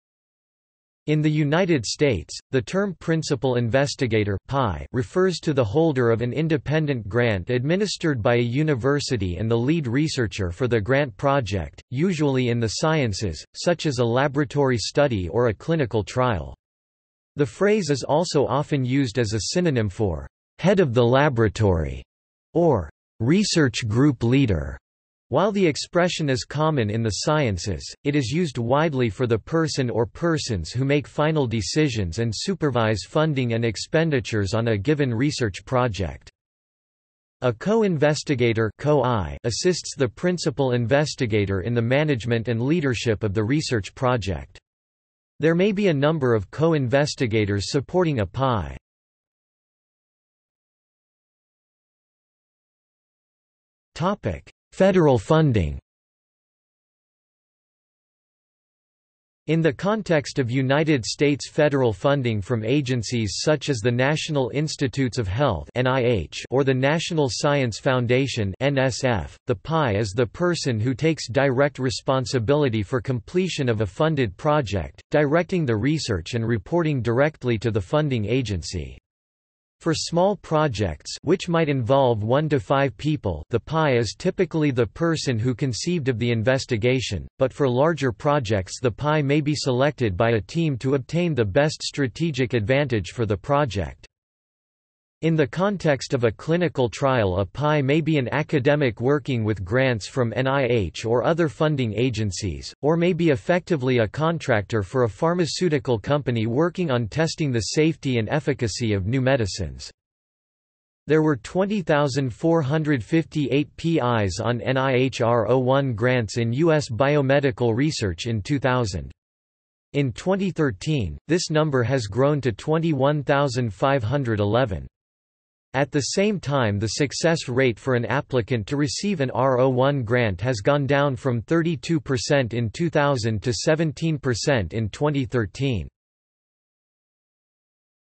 Principal Investigator | Wikipedia audio article
00:00:50 Federal funding

- Socrates

SUMMARY
In the United States, the term principal investigator (PI) refers to the holder of an independent grant administered by a university and the lead researcher for the grant project, usually in the sciences, such as a laboratory study or a clinical trial. The phrase is also often used as a synonym for "head of the laboratory" or "research group leader." While the expression is common in the sciences, it is used widely for the person or persons who make final decisions and supervise funding and expenditures on a given research project.
A co-investigator (Co-I) assists the principal investigator in the management and leadership of the research project. There may be a number of co-investigators supporting a PI.